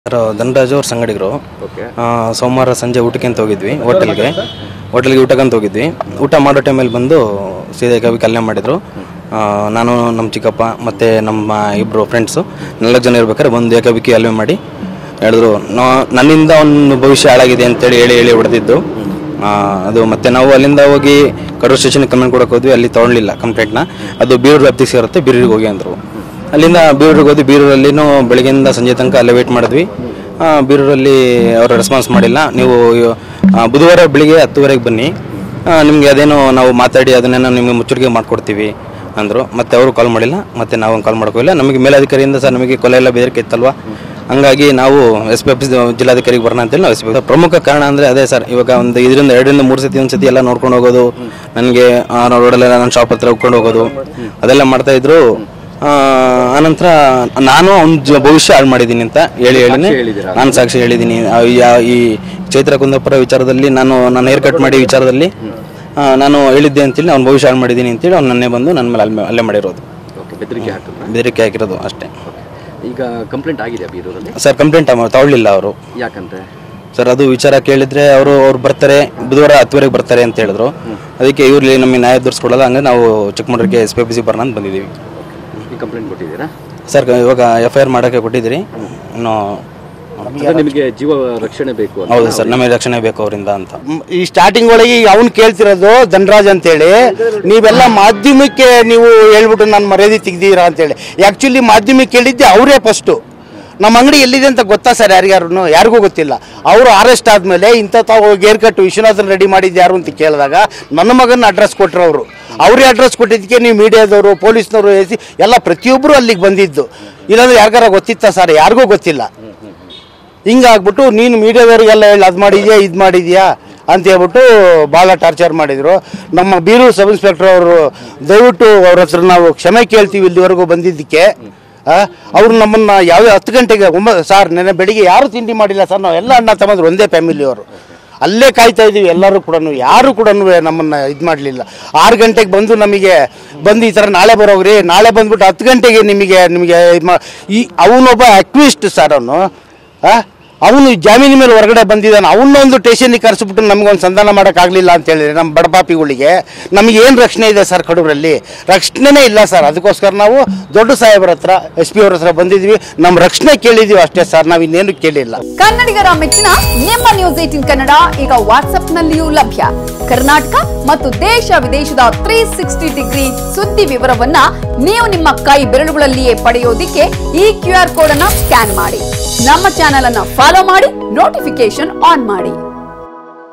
Chaitra Kundapura Case | ಚೈತ್ರಾ ಹೆಸರಲ್ಲಿ ಧನರಾಜ್ ಭವಿಷ್ಯ ಹಾಳು ಮಾಡಿದ್ದೇನೆ ಅಂತ ಹೊಡೆದ್ರು
Chaitra Kundapura Case | ಚೈತ್ರಾ ಹೆಸರಲ್ಲಿ ಧನರಾಜ್ ಭವಿಷ್ಯ ಹಾಳು ಮಾಡಿದ್ದೇನೆ ಅಂತ ಹೊಡೆದ್ರು  | N18V

News18 Kannada Live | ನ್ಯೂಸ್ 18 ಕನ್ನಡ ಲೈವ್
Karnataka | ಕರ್ನಾಟಕ
Kannada | ಕನ್ನಡ